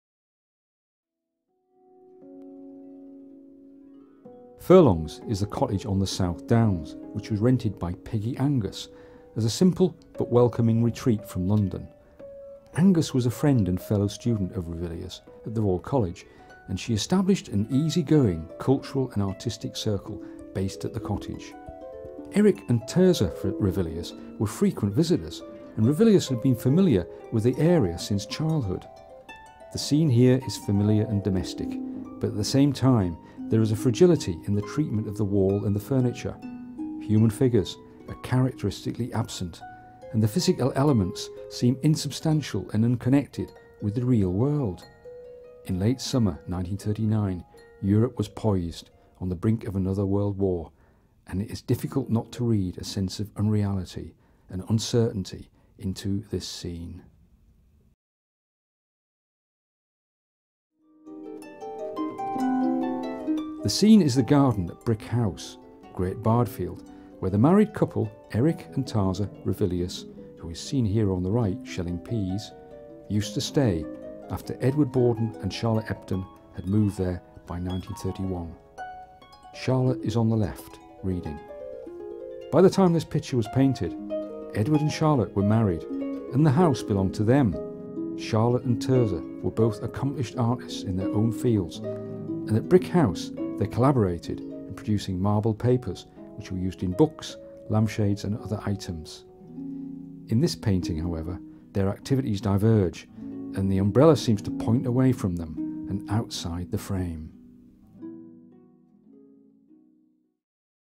Eric Ravilious at The Fry Art Gallery
The Fry Art Gallery has one of the most comprehensive collections of work by Eric Ravilious, including four of his  scrapbooks